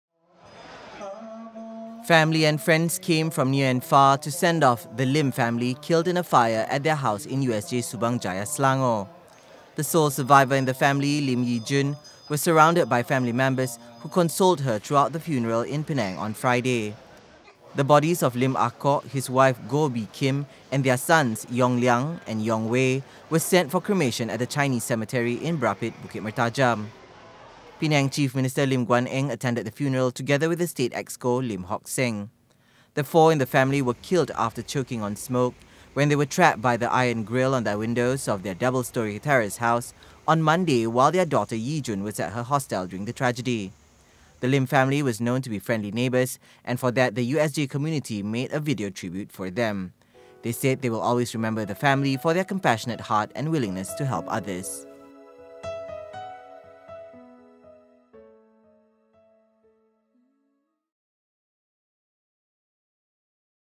Heart-wrenching sendoff for family killed in fire
A family of four killed in a fire in their home received a warm sendoff by their family and friends in Penang on Friday.